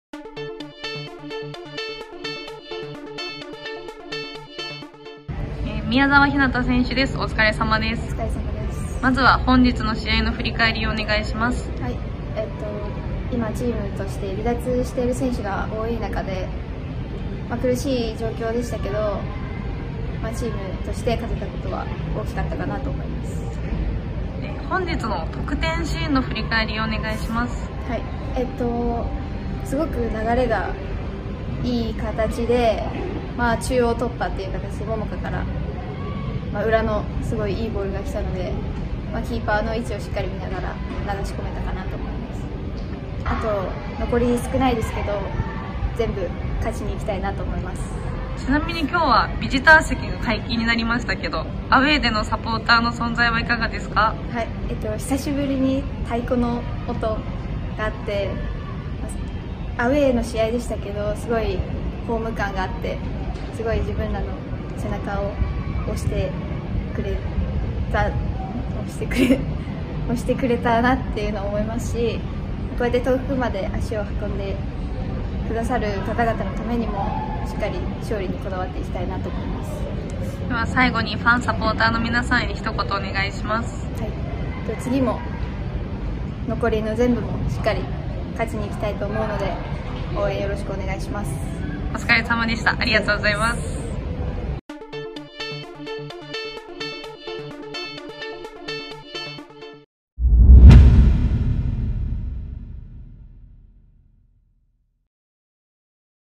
【Beleza TV】千葉L戦／宮澤ひなた 選手試合後インタビュー
[MATCH MOVIE] after the match against JEF UNITED ICHIHARA CHIBA LADIES

ジェフレディース戦、今季6ゴール目を決めた、宮澤ひなた選手の試合後インタビューをお届けします！

◆2020プレナスなでしこリーグ1部 第9節
ジェフユナイテッド市原・千葉レディース 1-3 日テレ・東京ヴェルディベレーザ